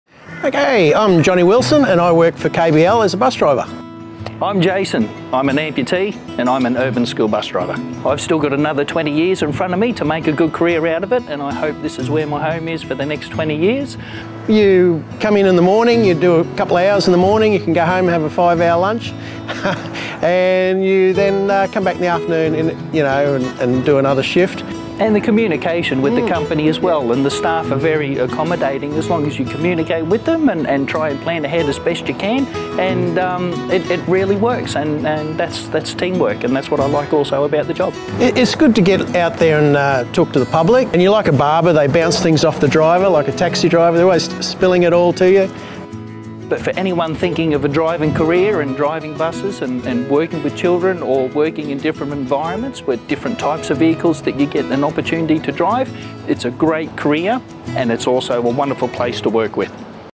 Enjoy great flexibility as a Kangaroo Bus Lines urban and school bus driver
Our culture of People, Pride and Passion shapes our commitment to deliver exceptional customer service across our whole operation.

Interested in taking the next step in your career with an industry-leading family-owned business? Kangaroo Bus Lines has a job for everyone!